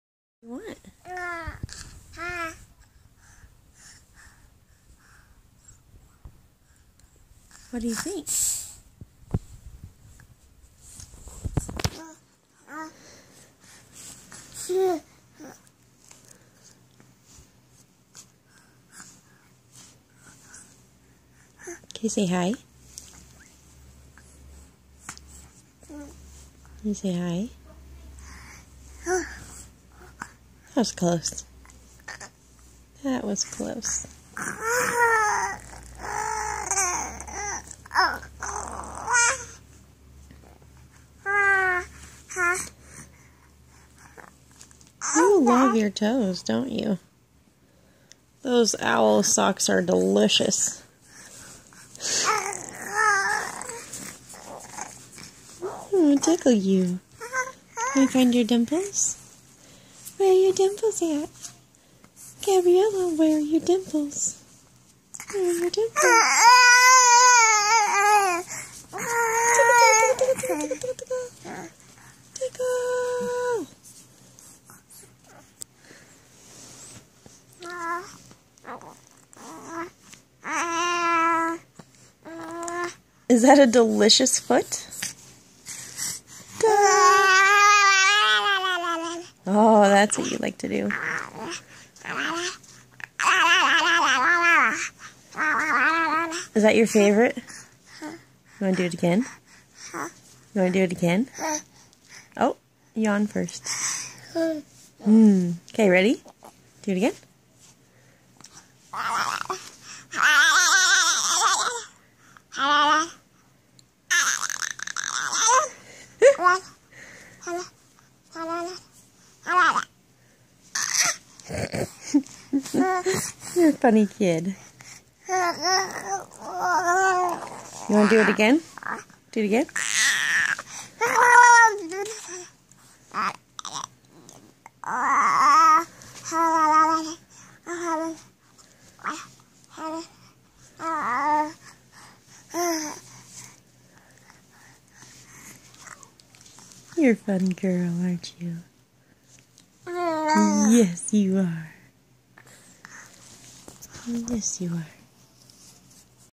Gabriella Rose 5 1/2 months old ♡
Just playing around with our toes